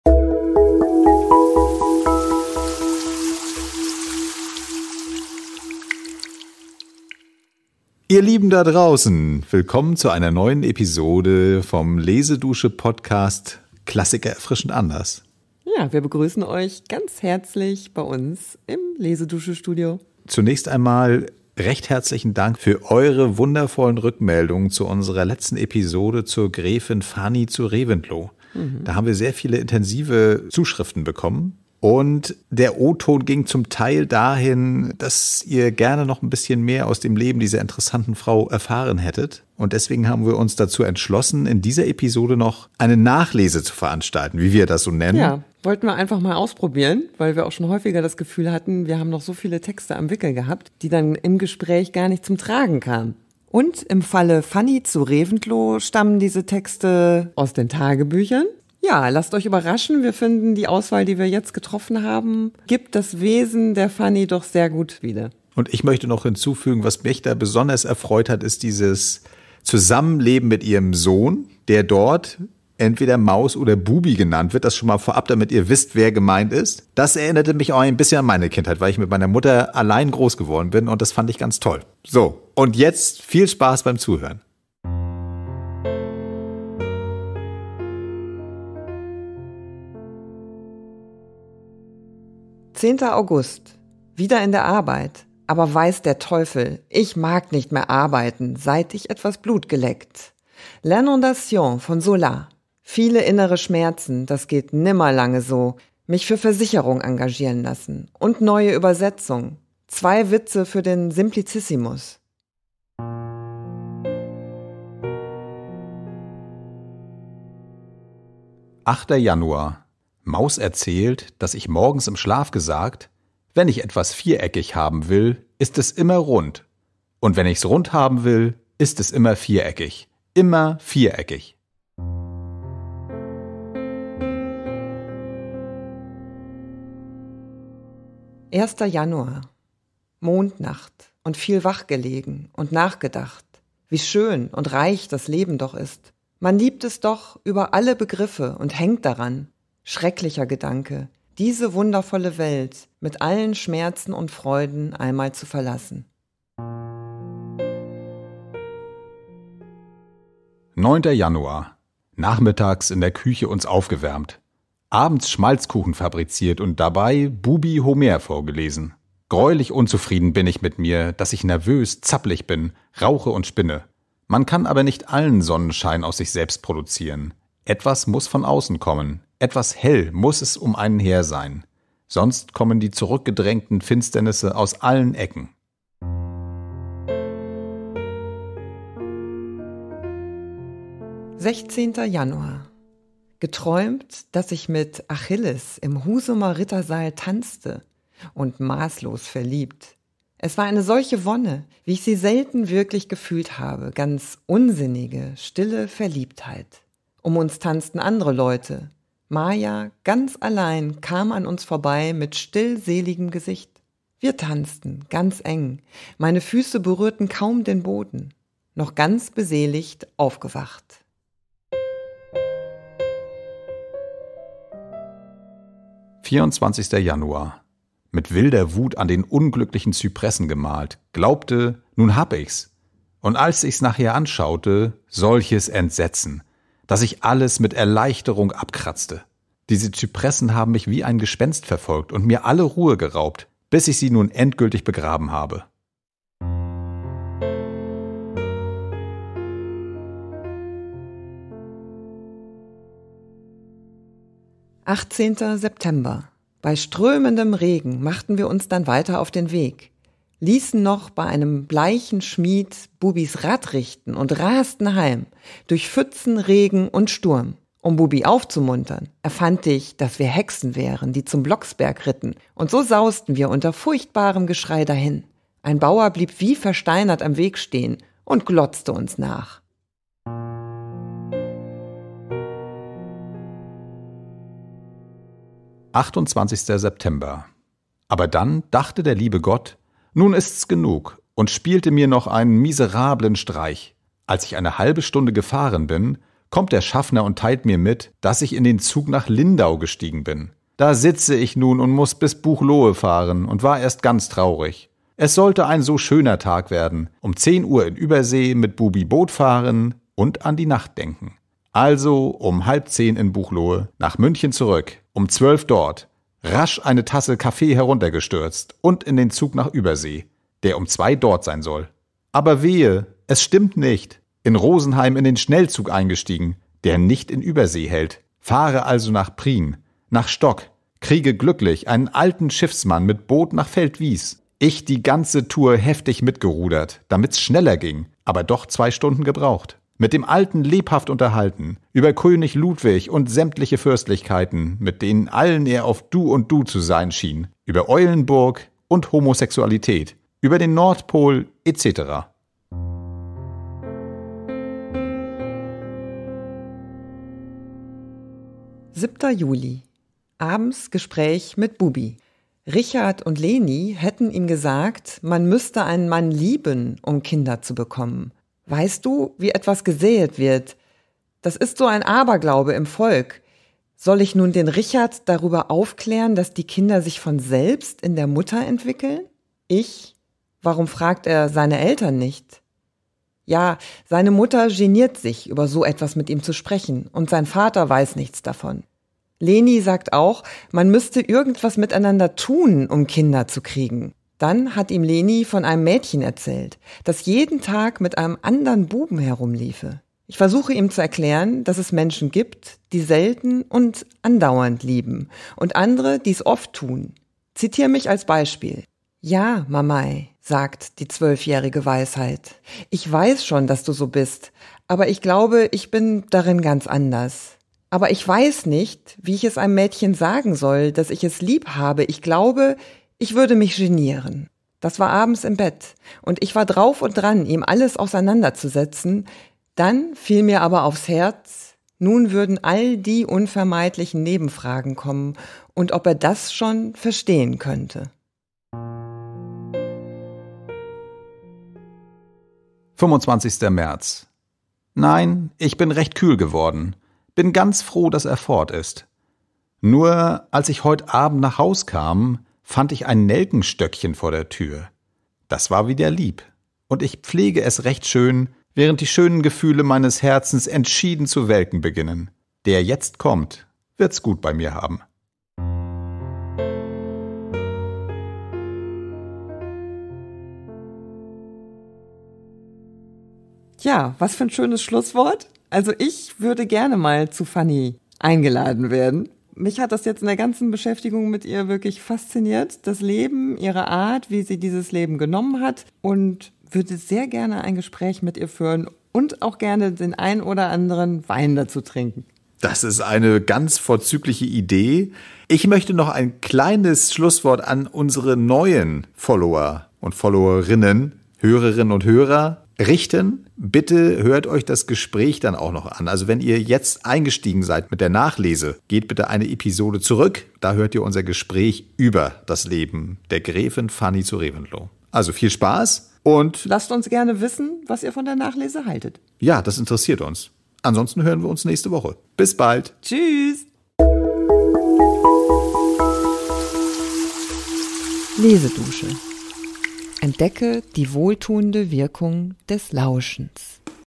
Aus Fanny zu Reventlows Tagebüchern (Zitate)
Die Resonanz auf unser Gespräch über Fanny Gräfin zu Reventlow (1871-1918) war so groß, dass wir uns in dieser Woche zu einer "Nachlese" im Podcast entschieden haben. Anhand von Tagebuchauszügen aus den Jahren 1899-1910 kannst du noch ein wenig Zeit mit Fanny verbringen. Sie erzählt so mitreißend, plastisch und auch lustig aus ihrem Leben – als wäre man direkt dabei!

Alle von uns ausgewählten Texte von Fanny zu Reventlow findest du in der Lesedusche

und erhalte exklusive BUCHTIPPS 📚 zu allen zukünftigen Podcast-Episoden.